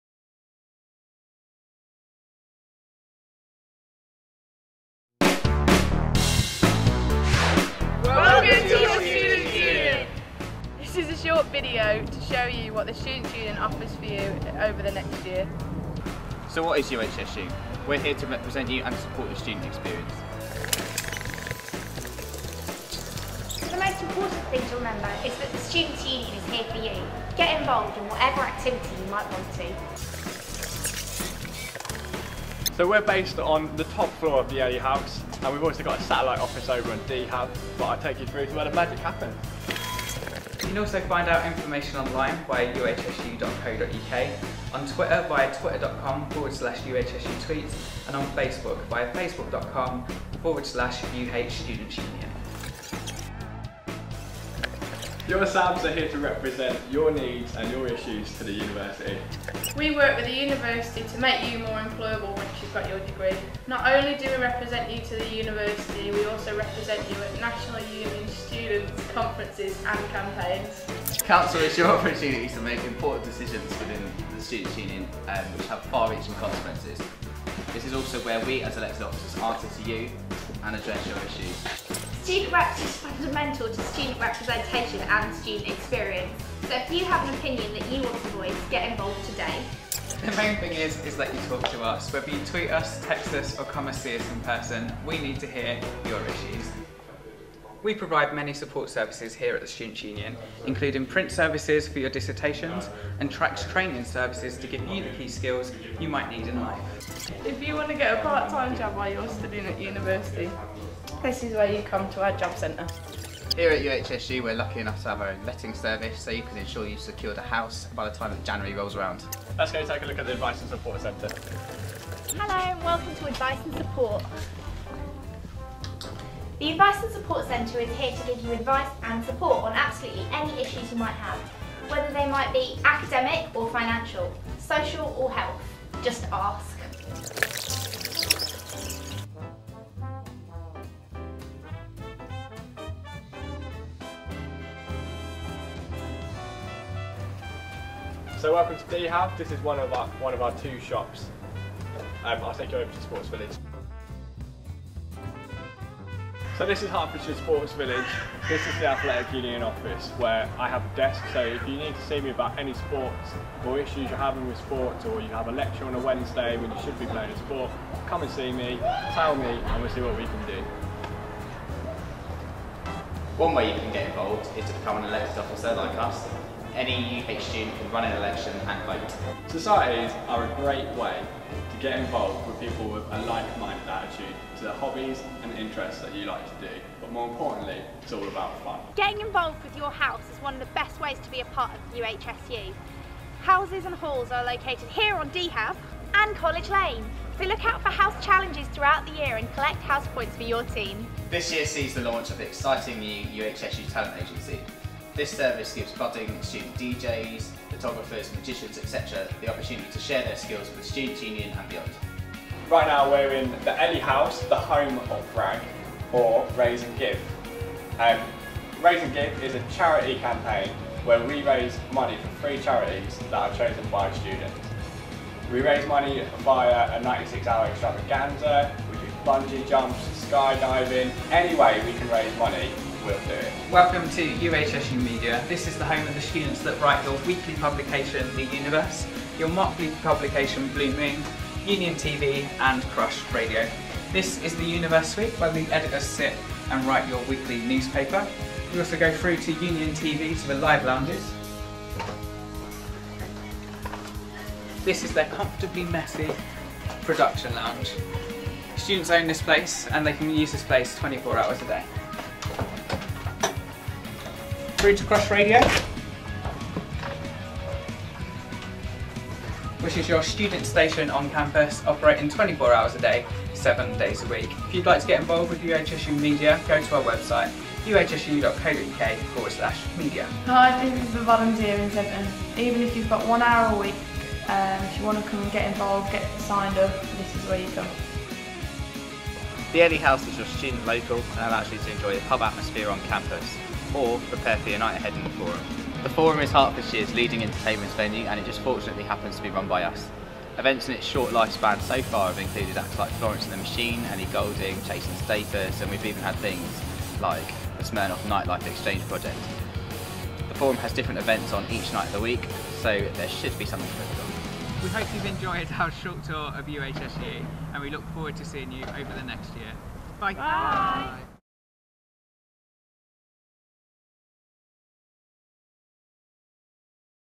UHSU Freshers 2011 - Welcome to UH!
Welcome to the University of Hertfordshire! In this video our sabbatical officers show you how to make the most out of your time as a student at UH.